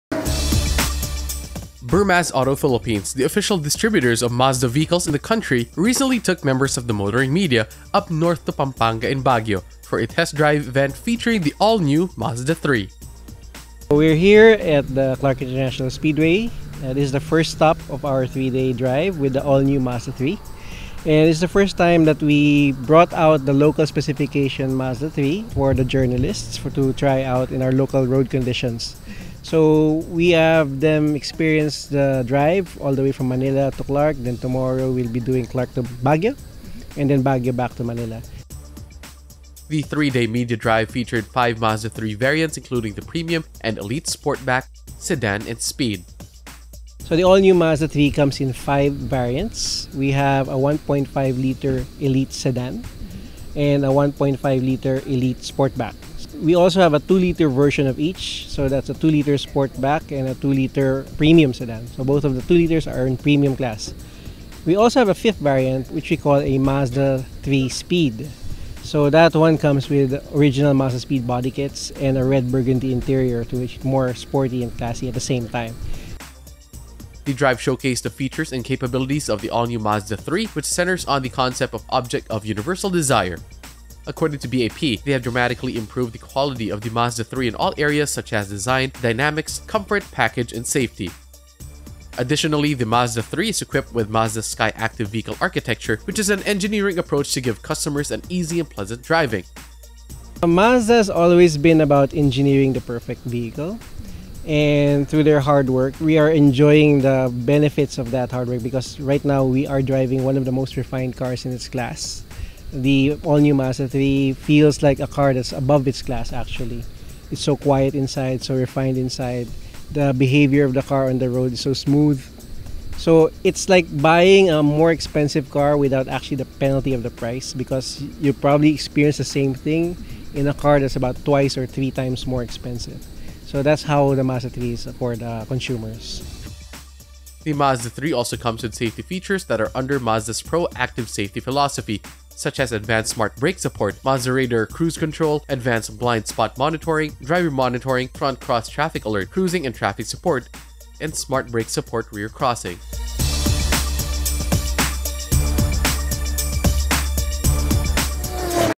Media Drive With The All-New Mazda3 | Industry News
Mazda Philippines recently took members of the motoring media up North to Pampanga and Baguio for a test drive event featuring the all-new Mazda3.